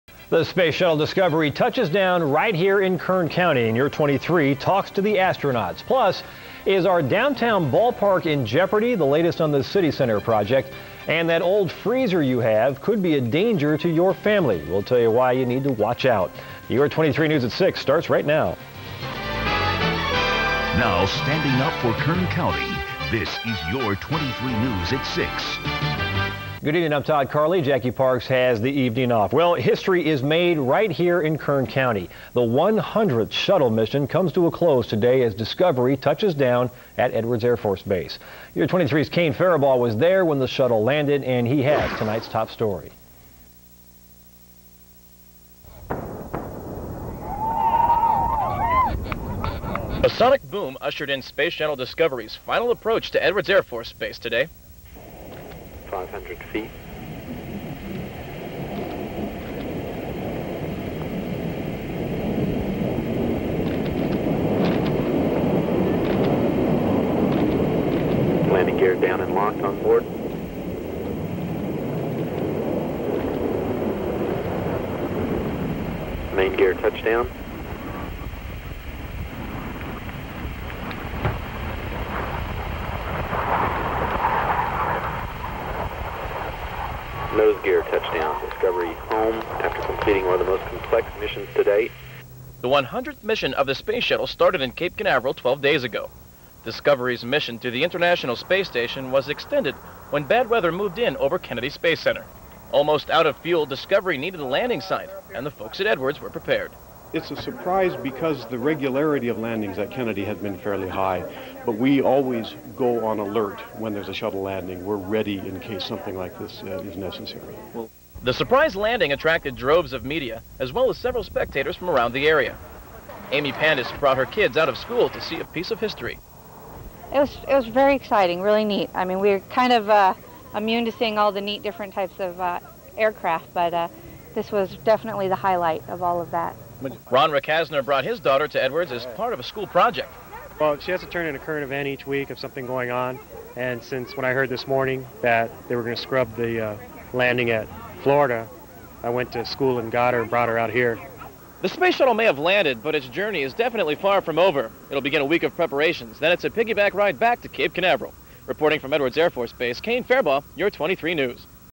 Space Shuttle Discovery STS 92 Landing Edwards Air Force Base
This is the story as I reported on the landing of the Space Shuttle Discovery at Edwards Air Force Base / Dryden Flight Center in California in 2000.  This was STS-92, the 100th mission of the Space Shuttle.  Bad weather had forced the shuttle to land at Edwards instead of Kennedy, the first time in more than ten years.  This story is from early in my career as a journalist, when I worked part-time at Your 23 News in Bakersfield, California.  I wish the quality was better on my original source tape.